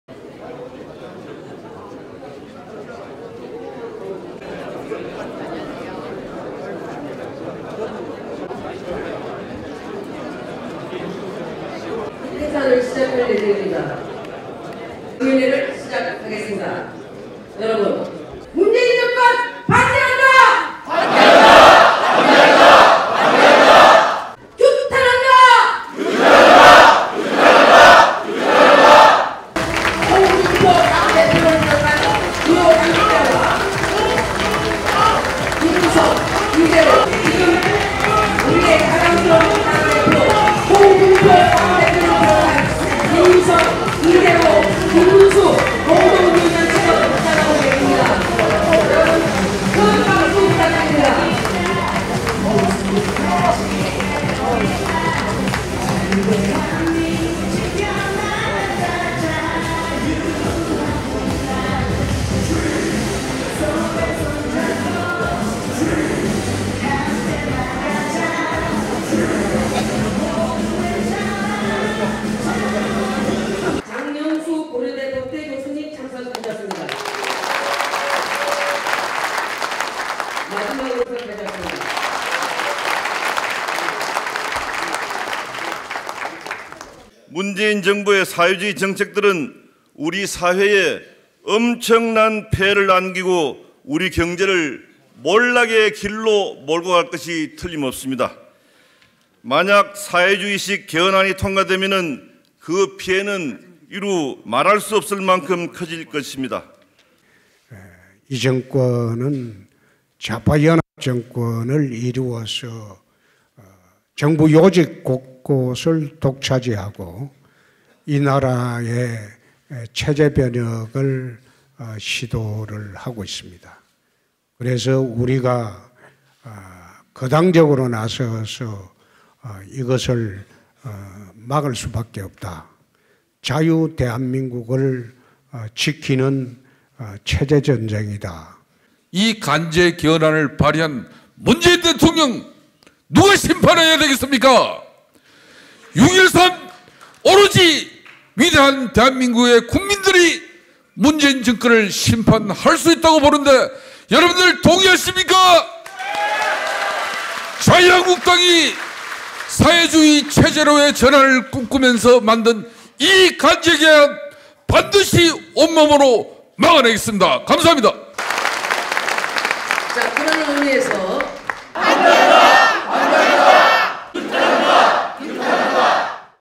자유한국당 개헌·정책 저지 시국 강연회
홍준표 자유한국당 대표와 참석자들이 17일 오전 국회 의원회관에서 열린 사회주의 개헌ㆍ정책 저지를 위한 대국민 시국강연회에서 결의를 다지고 있다.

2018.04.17 일요서울TV 장원용 기자